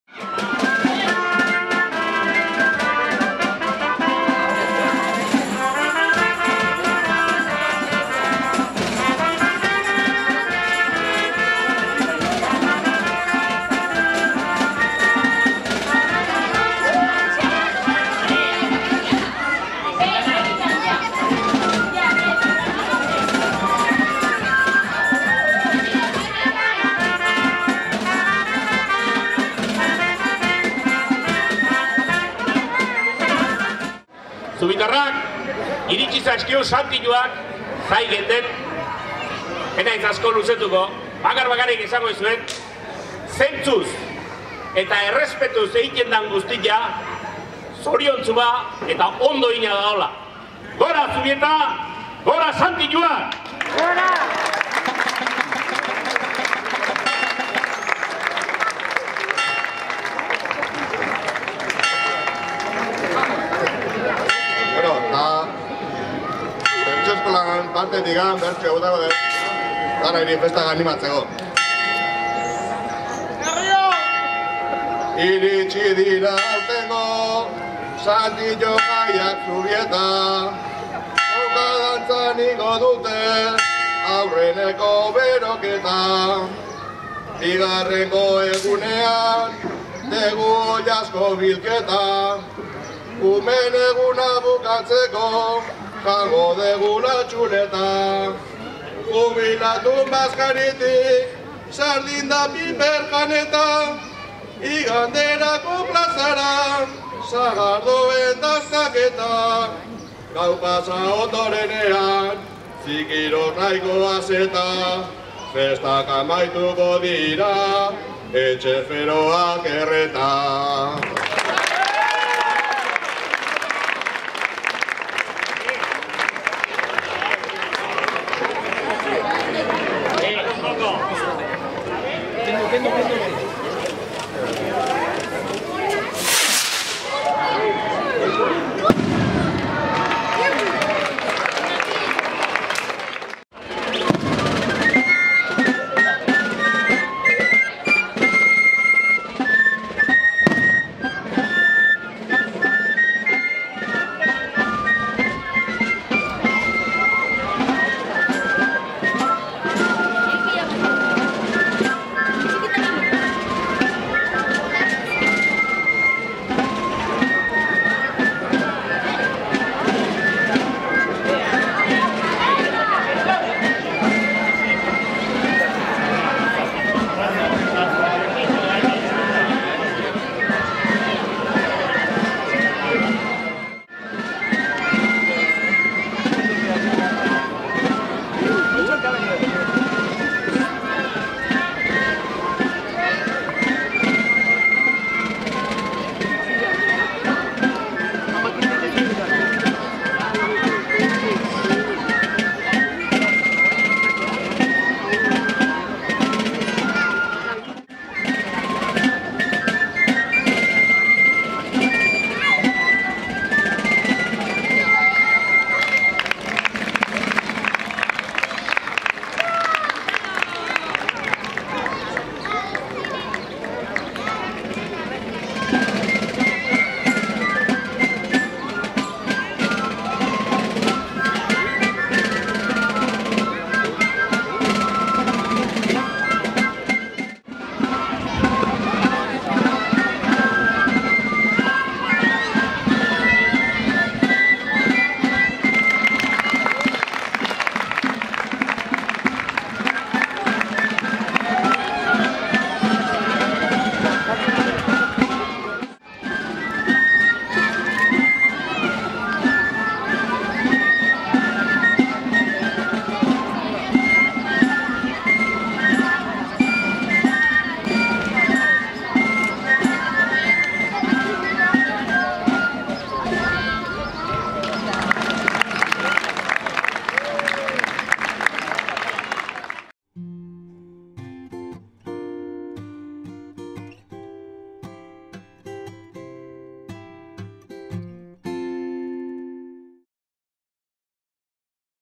Zubietako jaien hasiera
Santio Egunarekin abiatu dira Zubietako festak. Donostiako Txistulari Bandak girotu du eguerdiko soka-dantza.